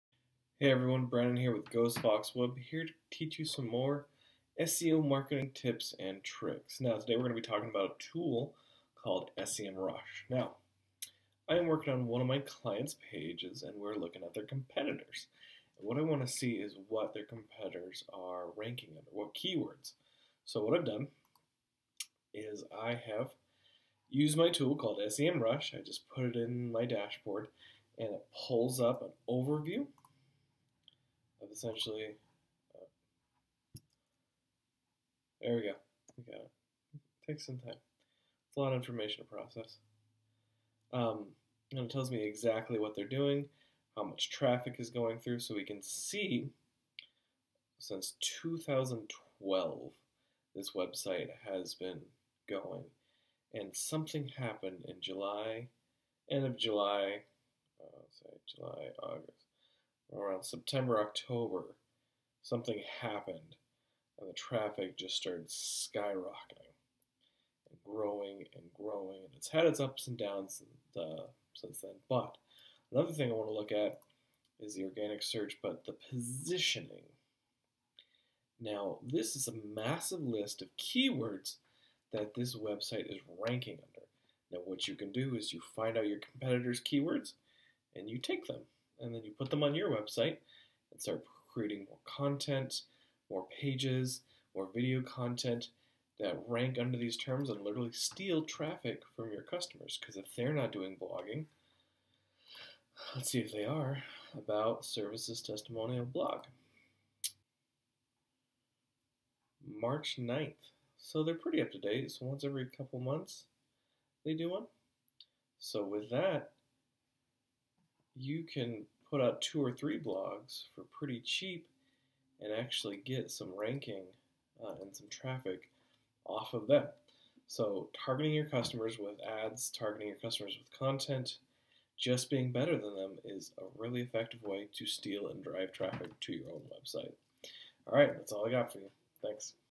How to Find Competitor Keywords In SEM Rush
If you are looking for great keywords to use and how to steal keywords from your competitors. SEM Rush is a great tool to use to get some organic keywords and get your website.